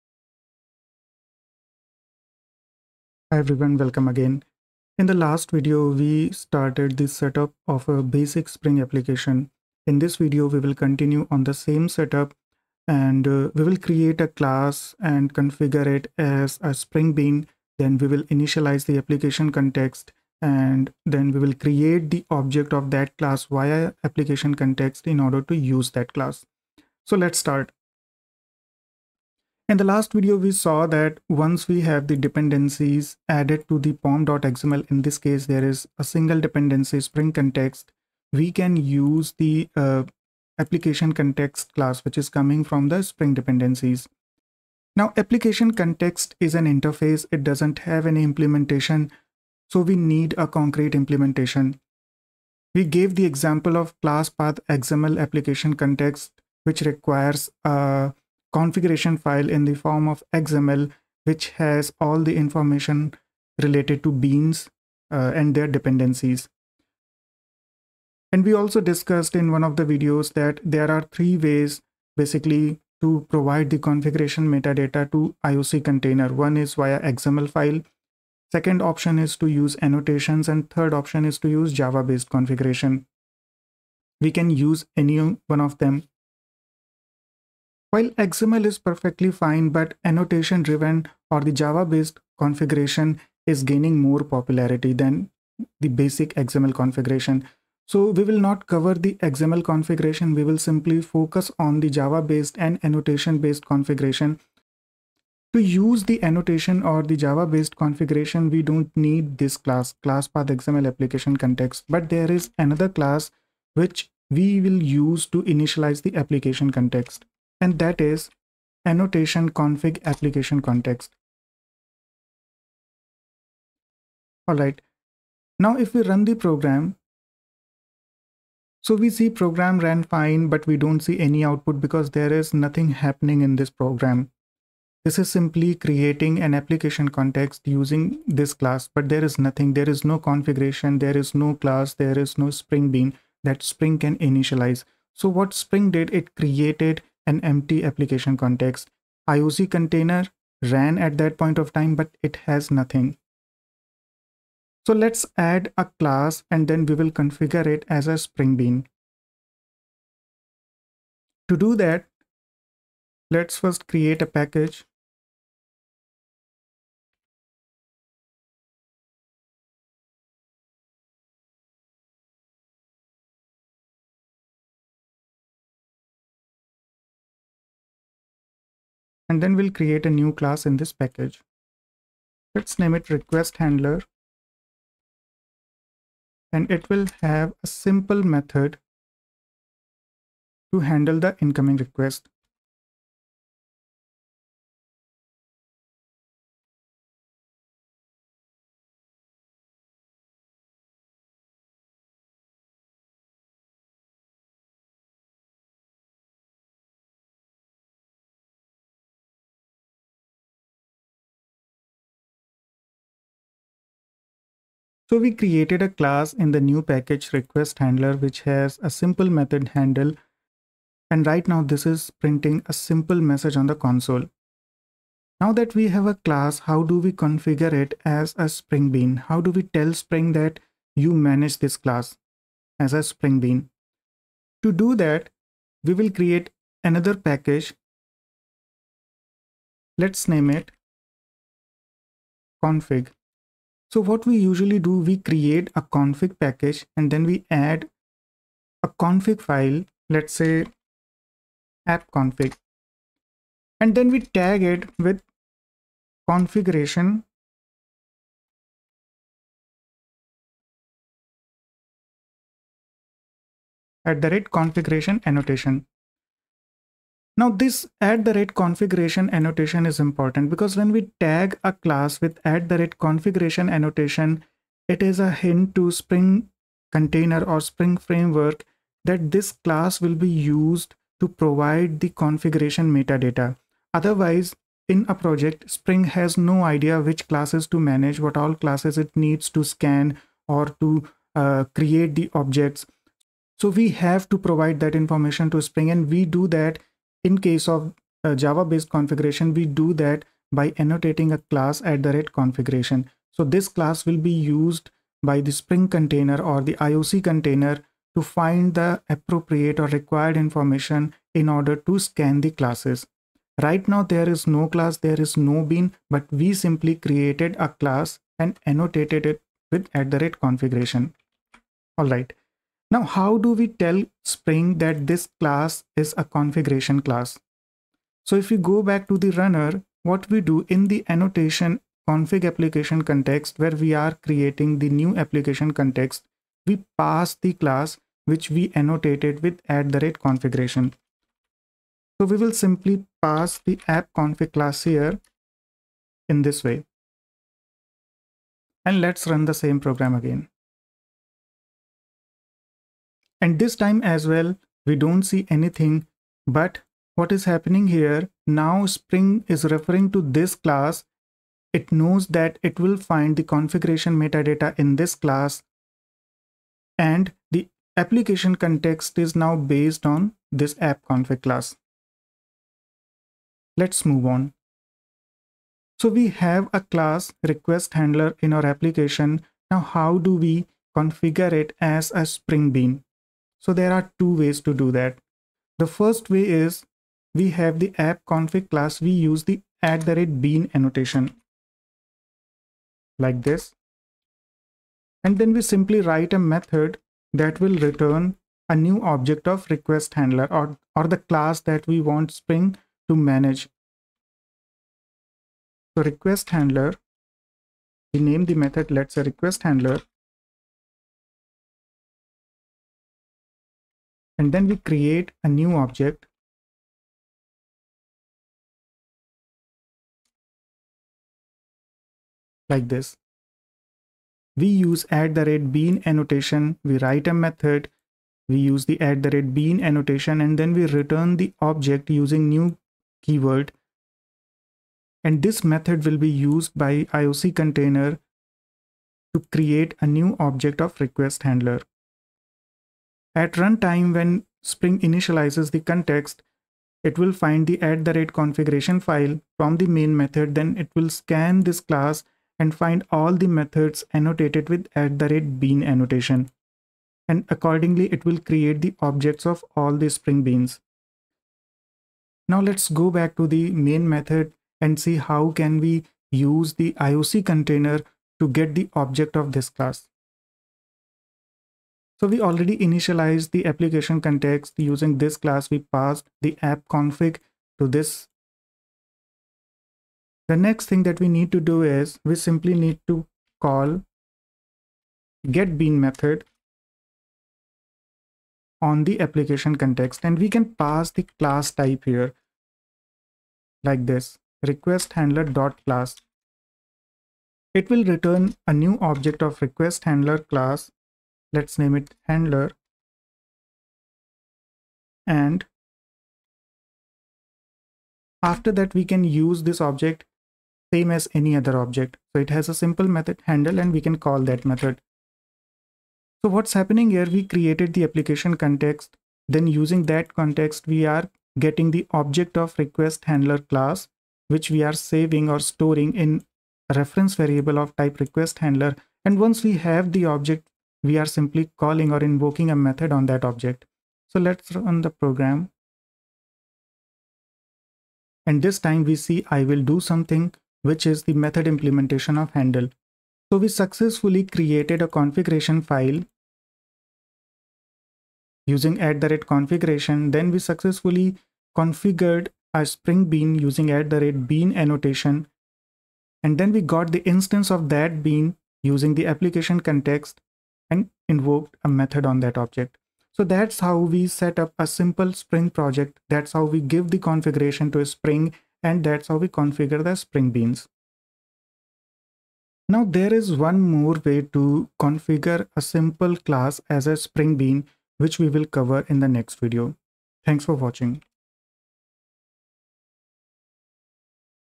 Configuring Spring Beans: Hands-On with @Configuration and @Bean Annotations | Part-6
Take a hands-on journey into the world of Spring Beans with our informative video tutorial! In this video, we dive deep into the powerful features of Spring Framework, specifically focusing on the configuration of Spring Beans using the @Configuration and @Bean annotations.

Join us as we provide a step-by-step demonstration, guiding you through the process of creating and configuring Spring Beans in your Java applications. We'll explore how the @Configuration annotation sets the stage for defining beans, and how the @Bean annotation allows you to customize bean instantiation and dependency injection.

By the end of this video, you'll have a solid understanding of how to leverage @Configuration and @Bean annotations to wire up your Java applications with Spring. Whether you're a beginner or an experienced developer looking to enhance your Spring skills, this video is for you.

So, grab your favorite IDE and get ready to dive into the world of Spring Beans. Watch now and unlock the power of configuring Spring Beans with @Configuration and @Bean annotations in your Java projects.

Share your thoughts and questions in the comments below. Let's embark on the journey of configuring Spring Beans together!